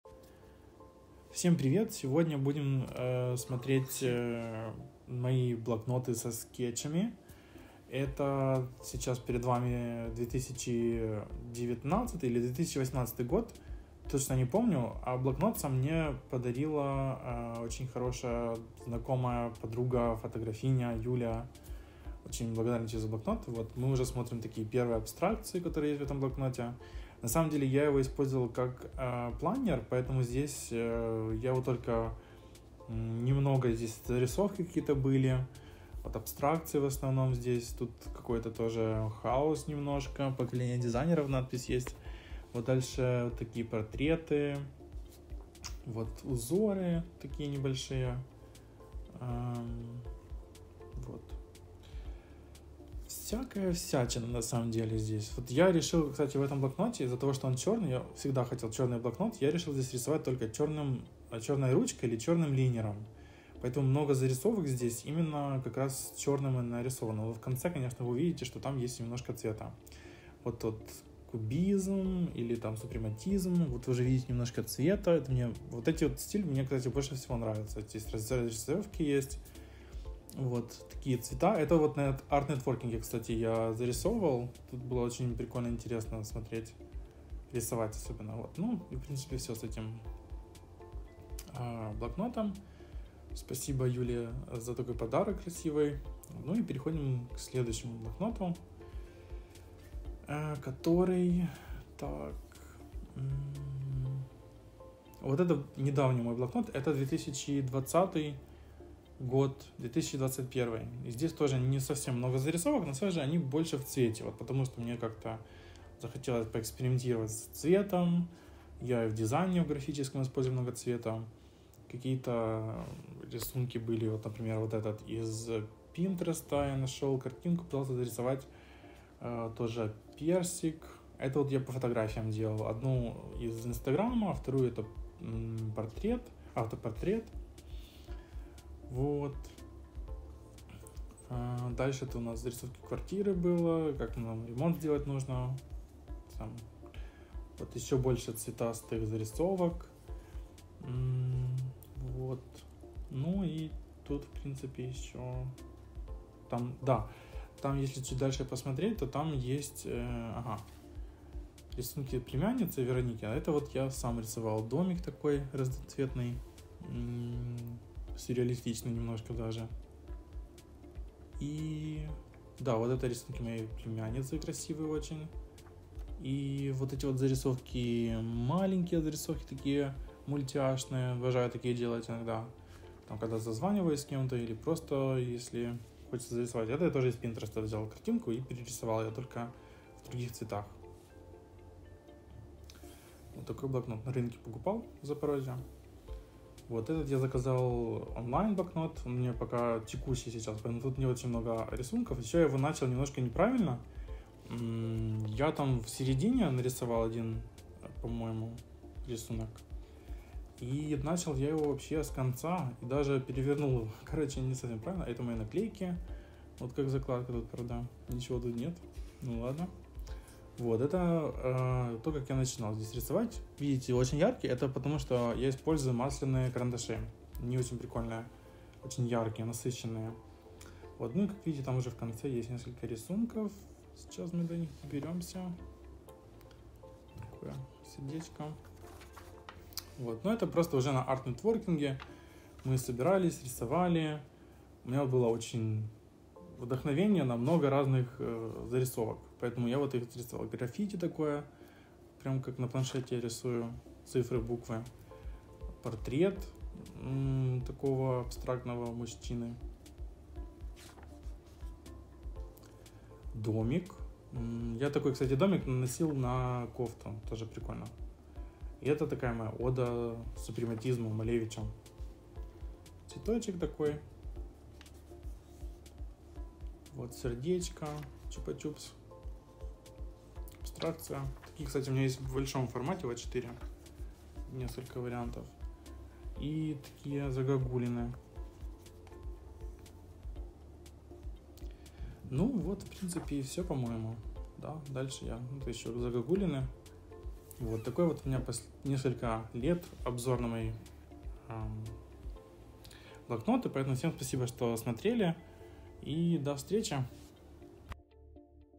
🎨📓MY SKETCHBOOK REVIEW 2018-2021 —зарисовки, супрематизм, импрессионизм, примитивизм
✦ОБЗОР СКЕТЧБУКОВ✦

Chapters
00:00 Начало
00:04 Первый скетчбук. 2018
01:42 Второй скетчбук. 2020
03:13 Третий скетчбук 2021